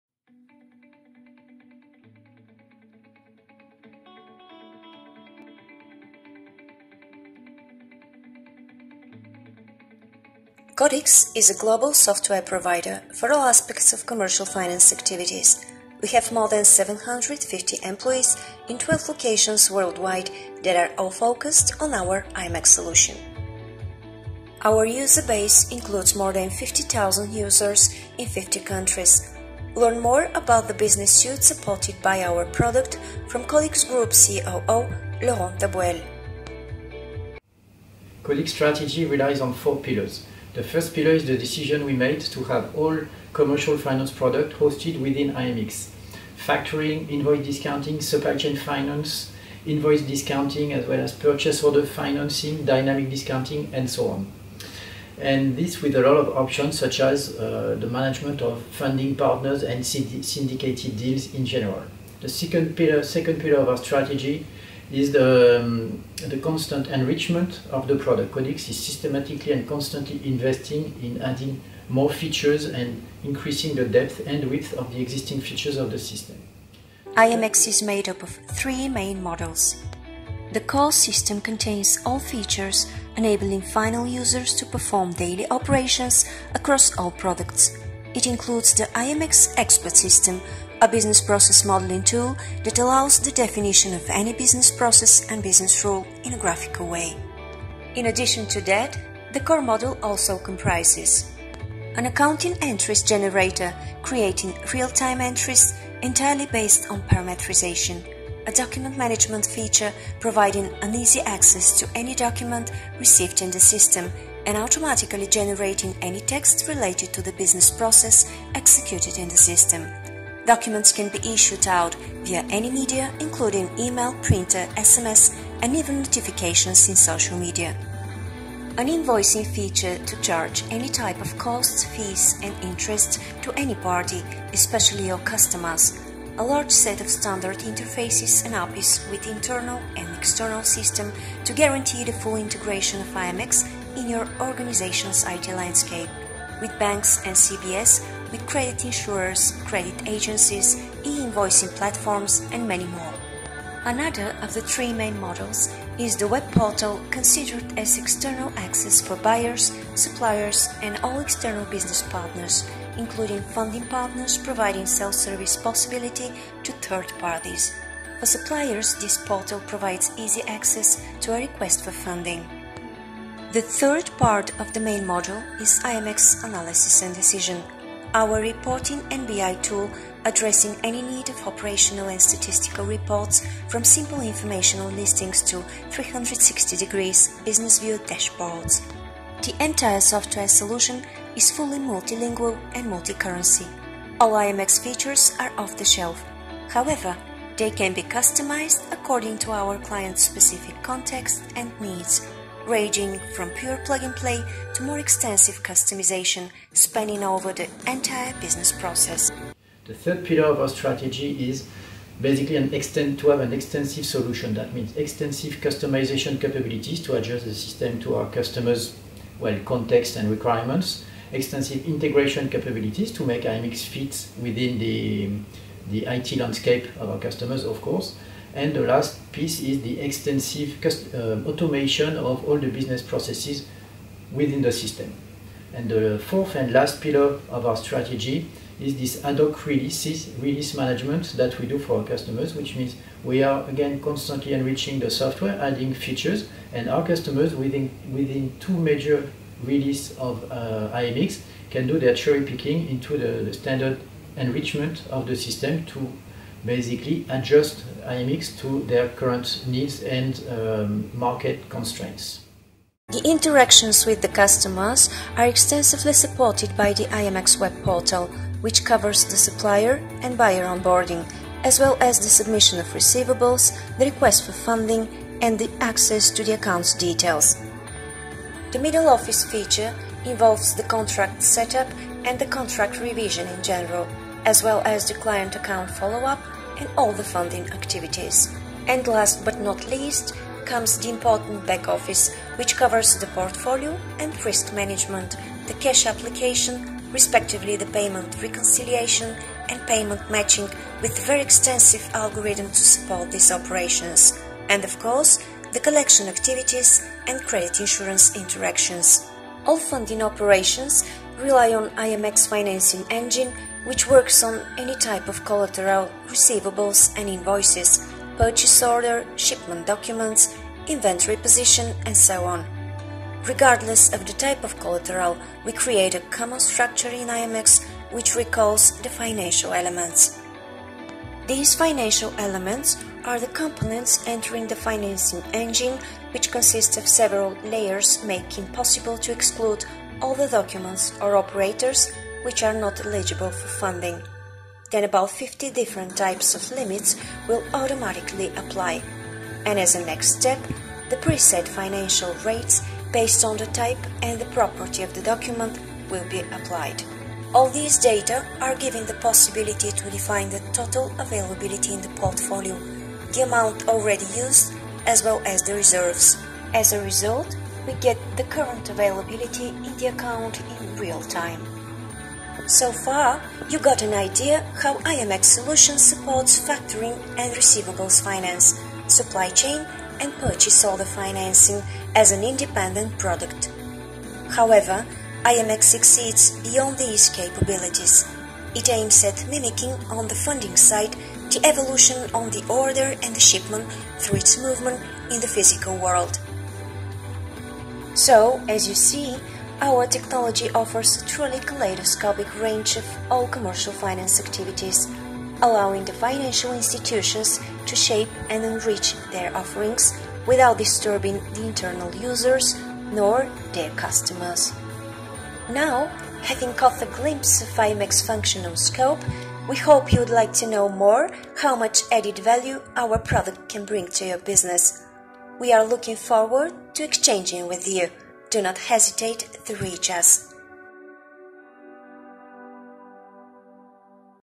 iMX Commercial Finance
iMX is an innovative blend of technologies, enabling all the business processes to be managed within a single technical structure that can easily be parameterized to meet each company’s specific needs. That makes it the leading software in several industries. iMX provides a complete functional coverage of the full lifecycle of all Commercial Finance products: receivables finance/factoring (domestic and international), payables finance/supply chain finance, inventory finance, selective invoice finance, ABL, etc. with unrivalled flexibility and depth in the system features.